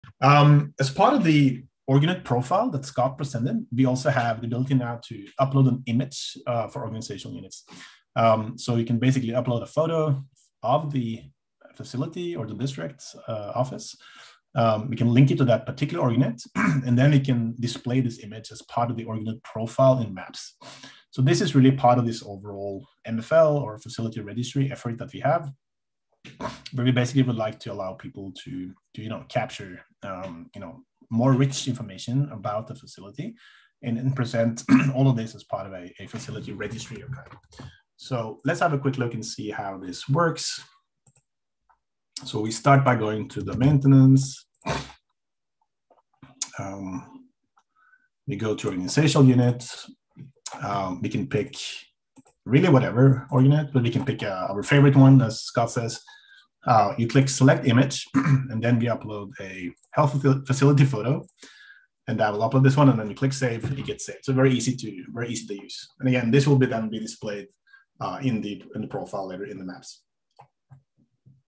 DHIS v2.37 Platform - Org Unit Image
Recording from a live demonstration of new features in DHIS2 version v2.37. This video demonstrates new Org Unit Image features.